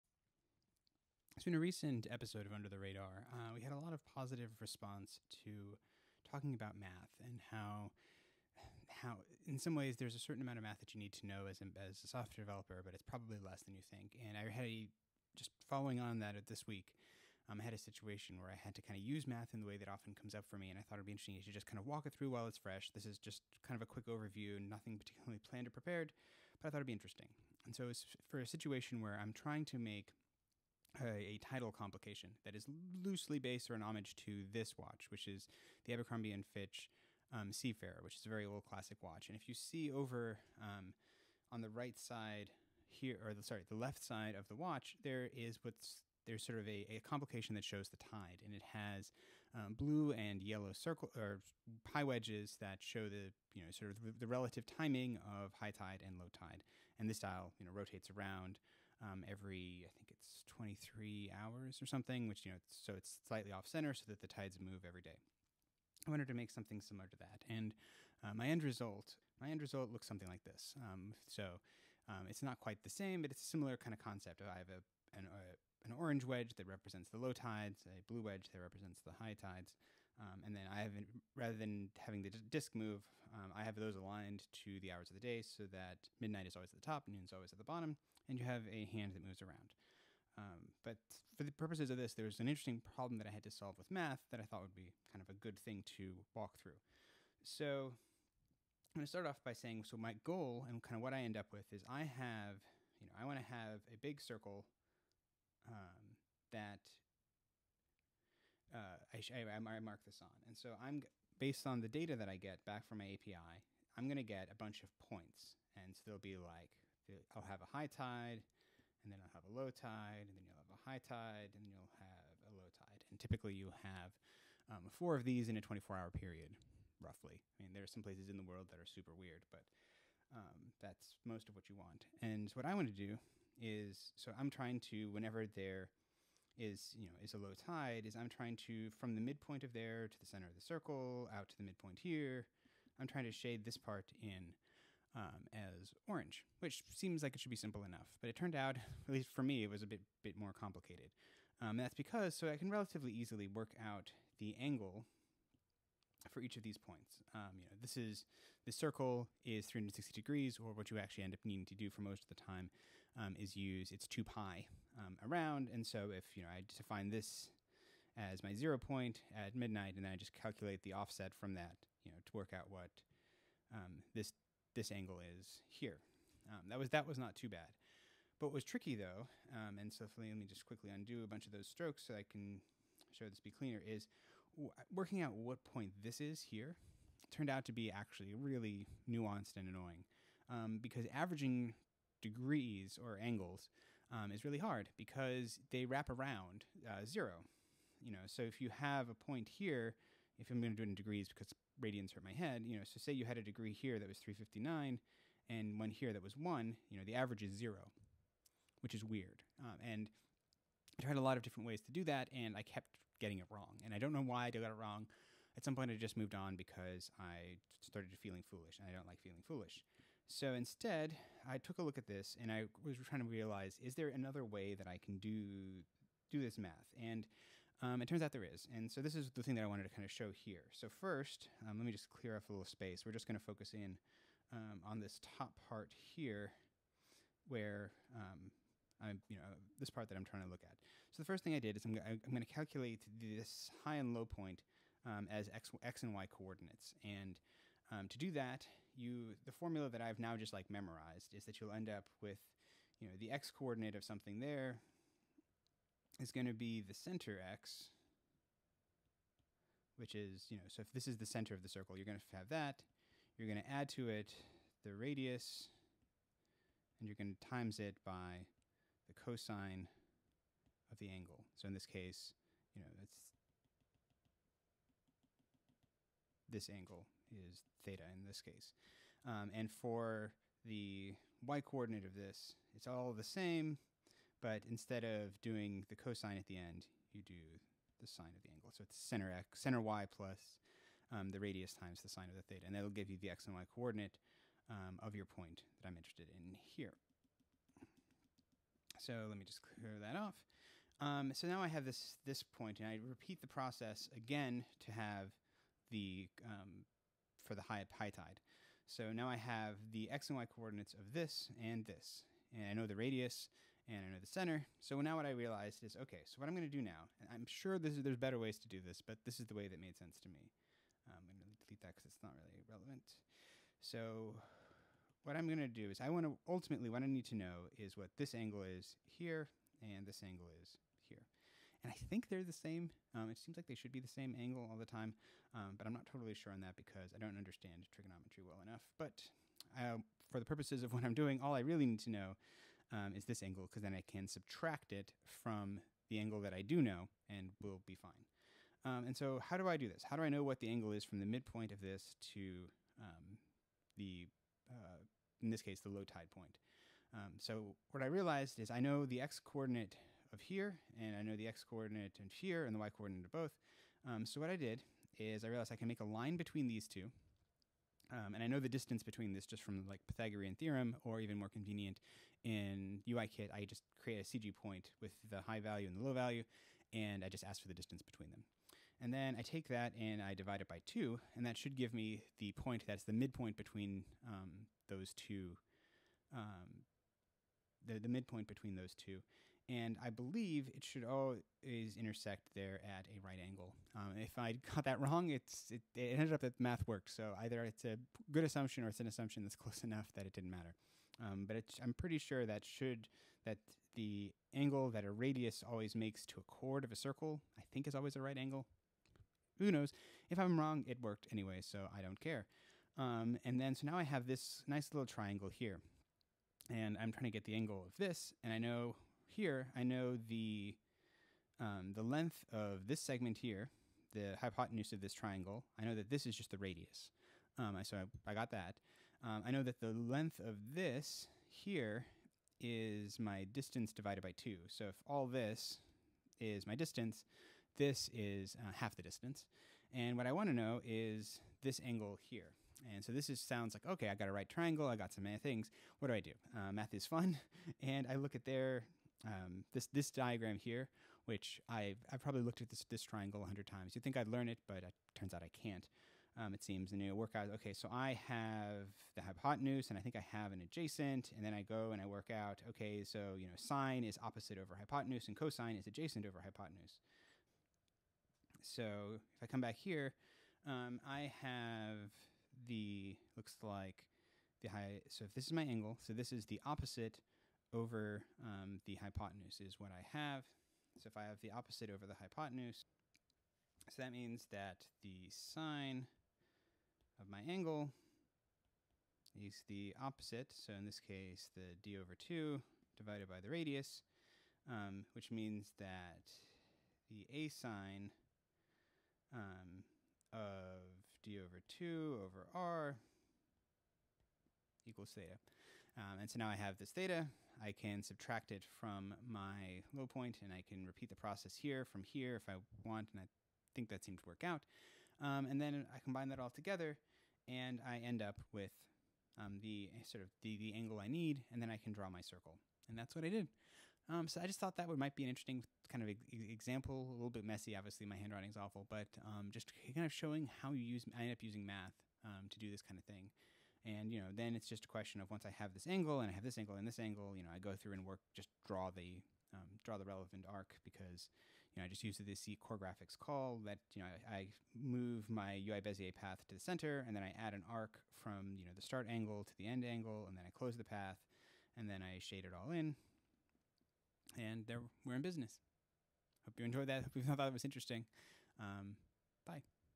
Math Involved in Drawing a Tide Complication
A quick, rough overview of the math I used to draw a Tidal complication.  Just trying to give an idea of what this process is like in practice.

Under the Radar episode talking about maths.